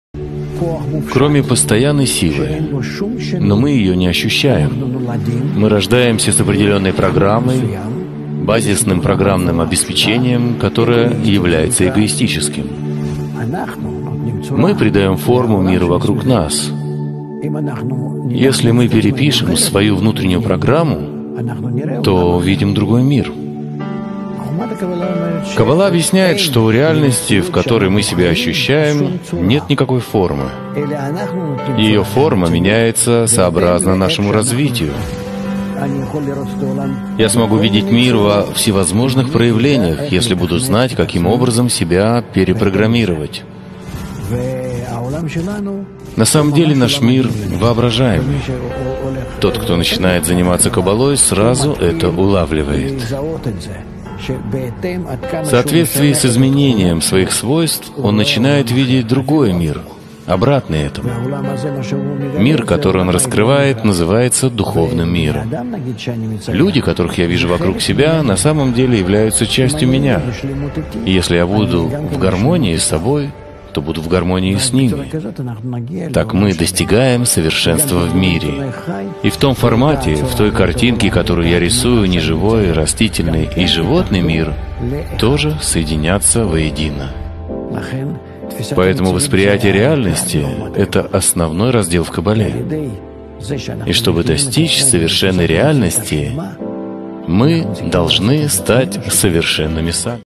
ВОСПРИЯТИЕ РЕАЛЬНОСТИ
Мы придаем форму миру вокруг нас. И поэтому, чтобы жить в другом мире, нам нужно переписать свою внутреннюю программу. Чтобы достичь совершенной реальности мы должны стать совершенными сами. И этому обучает нас наука Каббала.